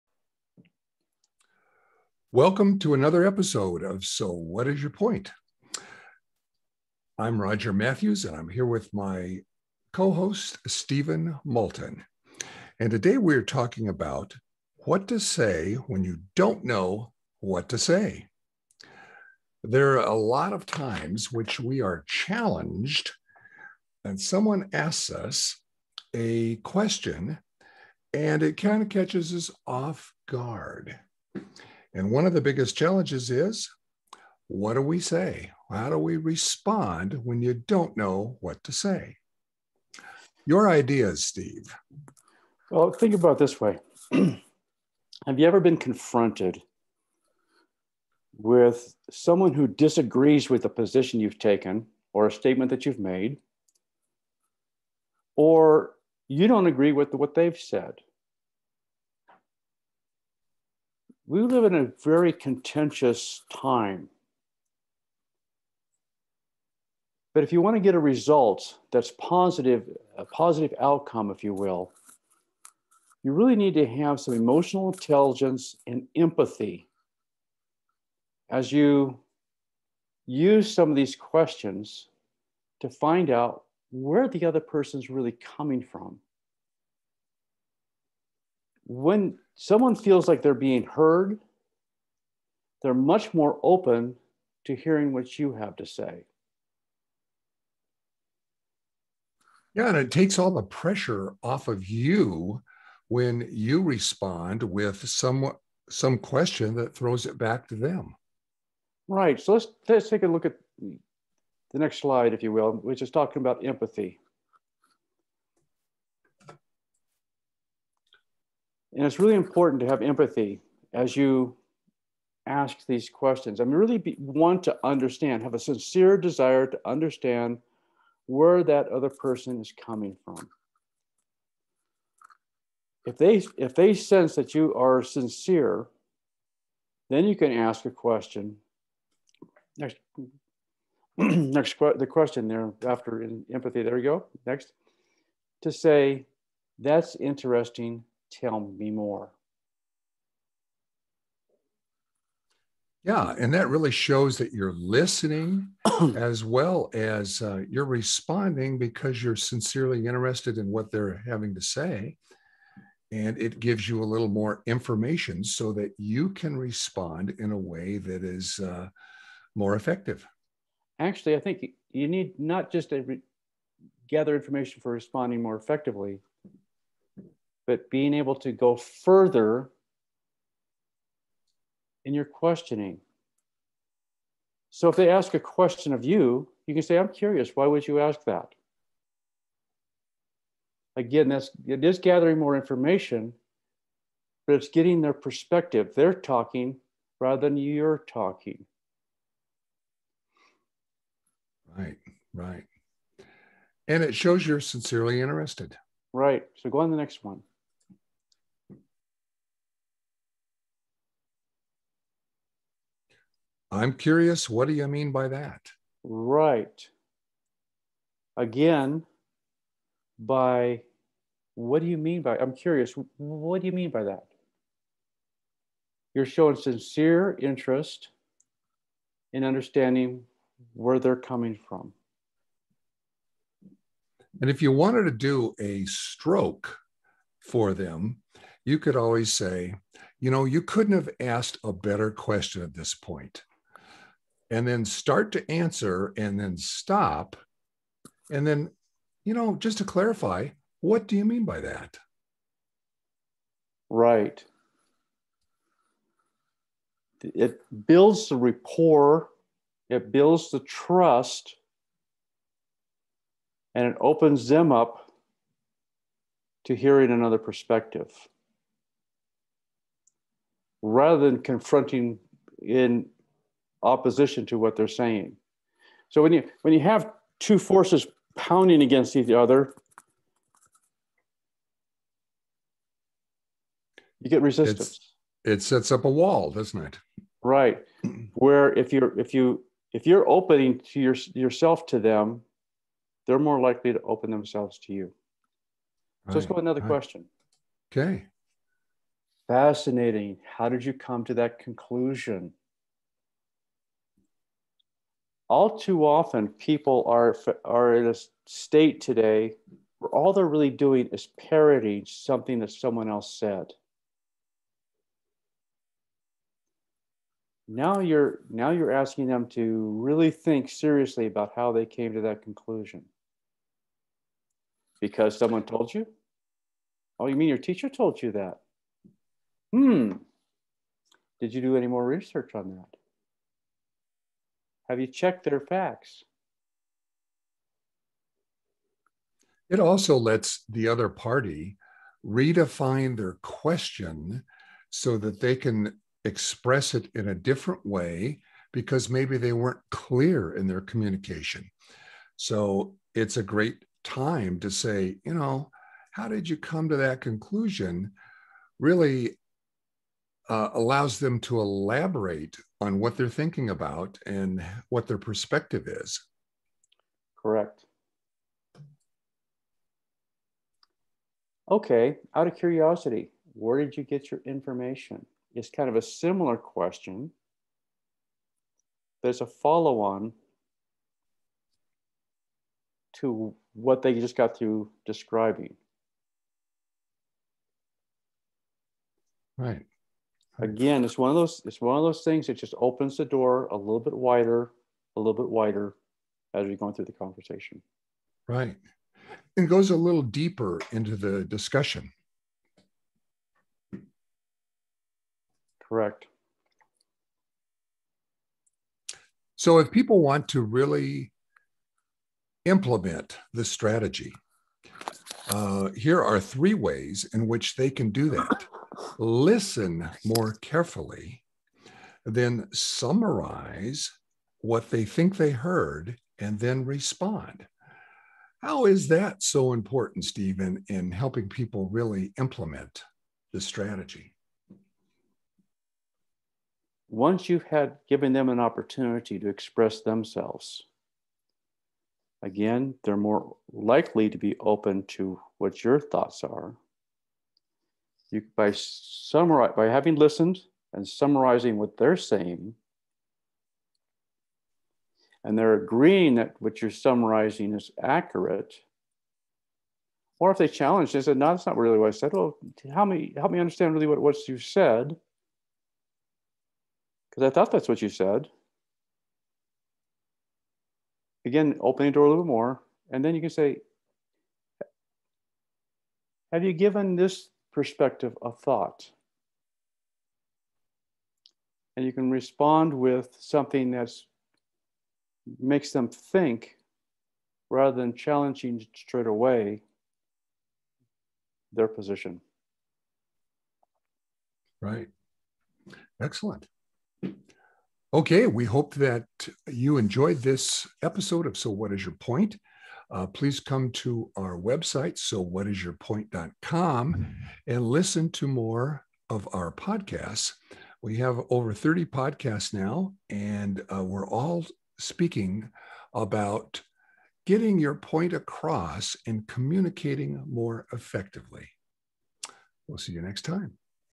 What To Say, When You Don't Know What To Say
Ever been caught off-guard or put on the spot and not known how to respond? It happens to everyone. Here are a few ideas to help you be prepared.